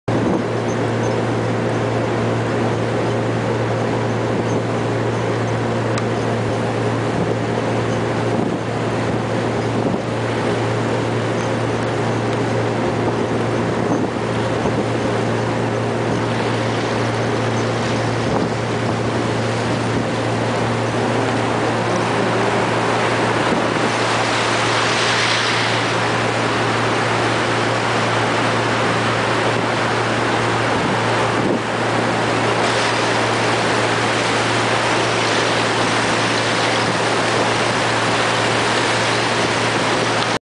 McKenzie River Ferry on Ice - Video 2
The ferry that took us over the McKenzie...unfortunately the ferry was still running with the ice breaker, and we weren't able to drive on the ice road.